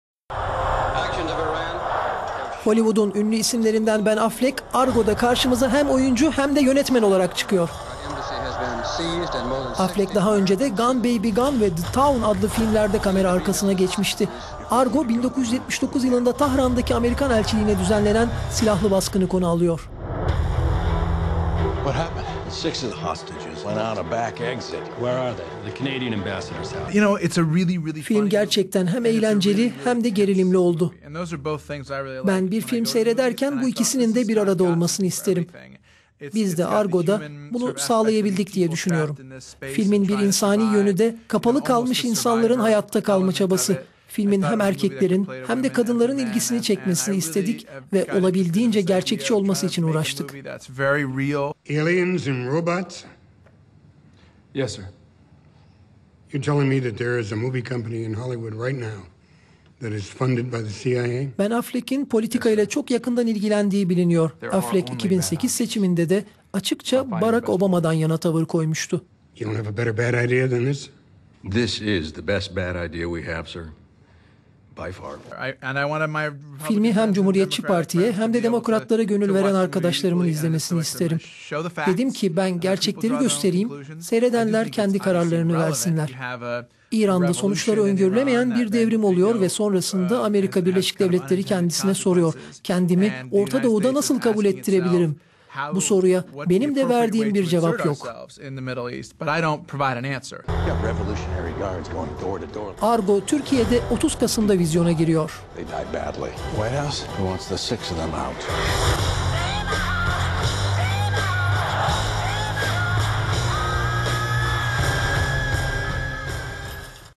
Obama taraftarı Affleck'ten seçimler öncesinde İran üzerine film: Argo
Hollywood sinemasının ünlü isimlerinden Ben Affleck Argo'da karşımıza hem oyuncu hem de yönetmen olarak çıkıyor. Affleck daha önce de Gone Baby Gone (2007) ve The Town (2010) adlı filmlerde kamera arkasına geçmişti.

"Argo" 1979 yılında Tahran'daki Amerikan elçiliğine düzenlenen silahlı baskını konu alıyor. Affleck John Goodman'la başrolünü paylaştığı filmde, rehineleri kurtarmak için Tahran'a gelen sahte bir film ekibinde yer alıyor. Filmin bazı sahneleri İstanbul'da Balat ve Eminönü'de de çekilmişti

"Filmin hem erkeklerin hem de kadınların ilgisini çekmesini ve olabildiğince gerçekçi olmasını istedik"
diyen Ben Affleck'in politikayla çok yakından ilgilendiği biliniyor. Affleck 2008 seçiminde açıkça Barack Obama'dan yana tavır koymuştu.

"Filmi hem Cumhuriyetçi Parti'ye hem de Demokratlar'a gönül veren arkadaşlarımın izlemesini isterim. Dedim ki ben gerçekleri göstereyim seyredenler kendi kararlarını versinler. İran'da sonuçları öngörülemeyen bir devrim oluyor ve sonrasında ABD kendisine soruyor: Kendimi Orta Doğu'da nasıl kabul ettirebilirim. Bu soruya benim de verdiğim bir cevap yok."

Film Türkiye'de 30 Kasım'da vizyona giriyor.